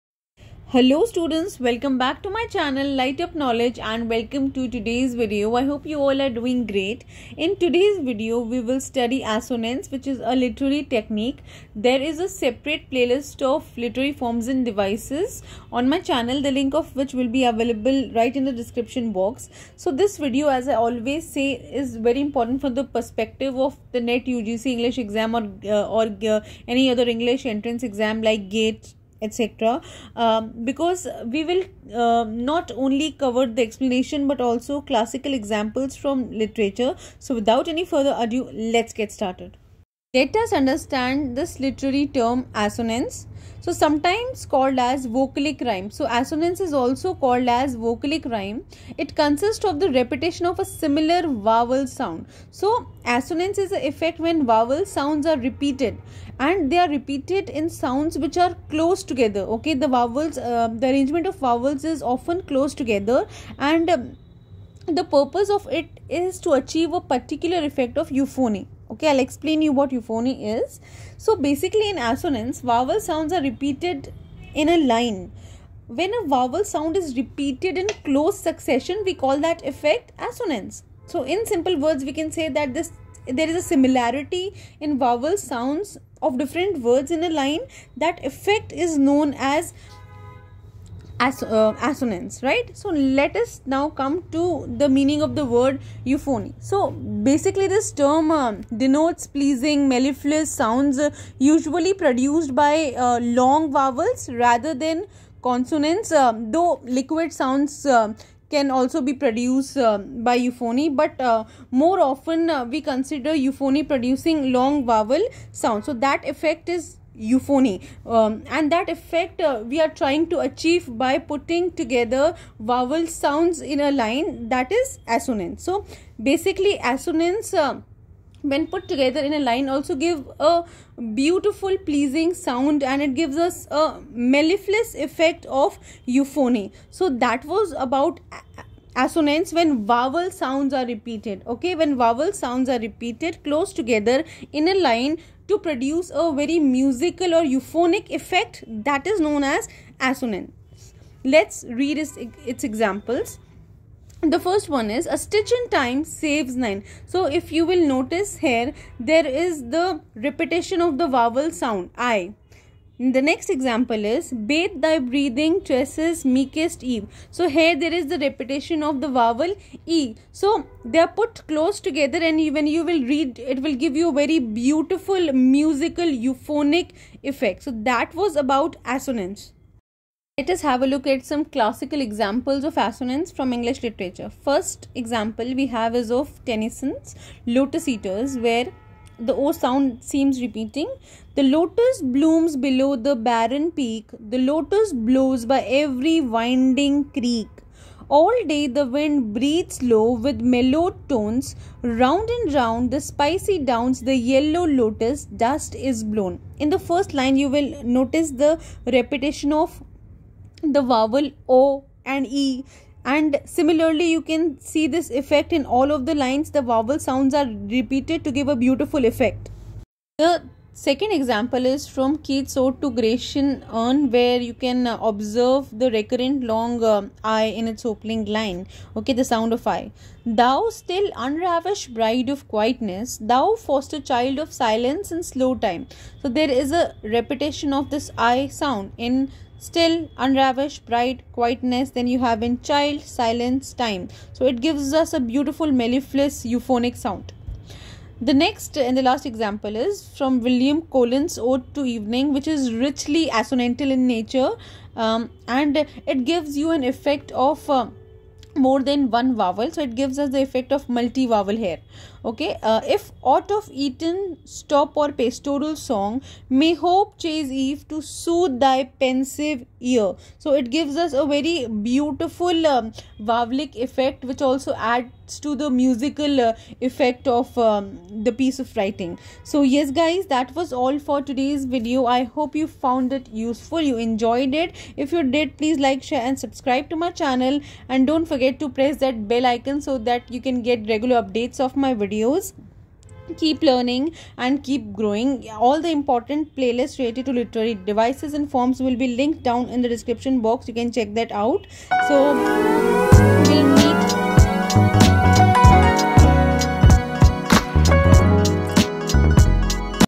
Assonance -A Literary technique
Hello students,  I hope you all are doing great. Sharing with you all a video lecture on Assonance .  I hope you find it useful.

Lightup Knowledge ❤